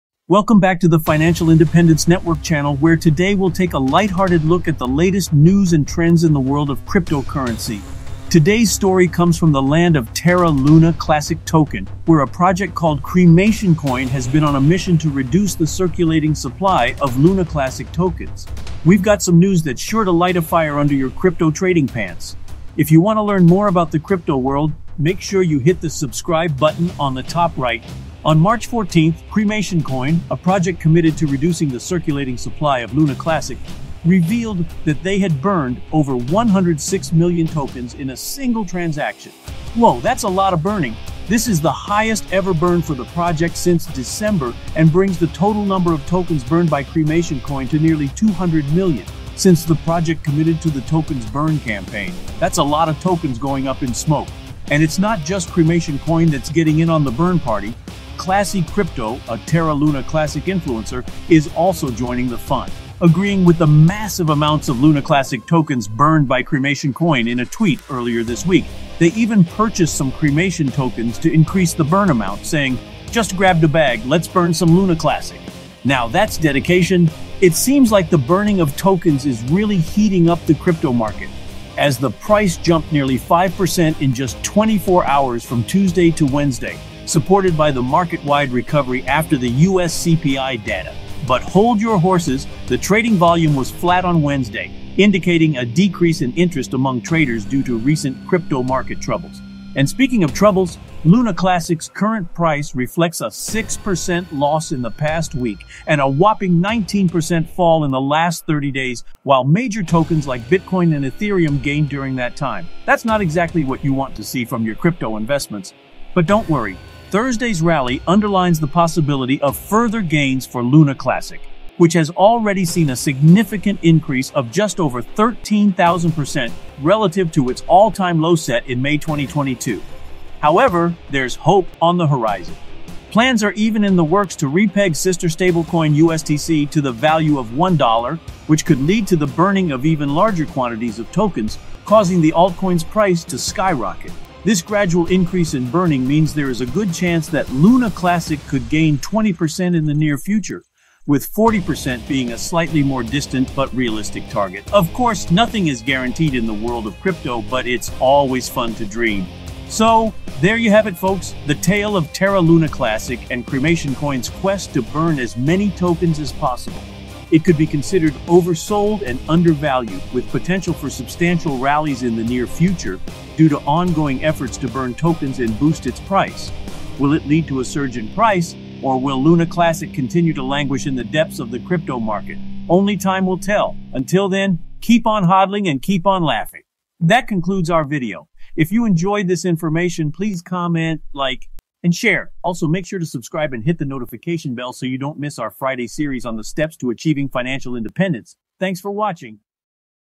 Terra Luna Classic (LUNC) Update!
In this video, we'll explore Terra Luna Classic (LUNC), a stablecoin-based cryptocurrency that's infamous in the crypto world. Built on the Terra blockchain, Luna Classic offers a range of DeFi services like staking, trading, lending, and borrowing.

Investing in Luna Classic is used to be a promising opportunity due to its solid reputation and growing user base. However, since it's drastic decline, we are starting to see the social sentiment start to increase again. Whether you're an experienced crypto investor or just starting out, join us as we take a closer look at Terra Luna Classic and what the future may hold. For you crypto lovers, this video is a MUST WATCH!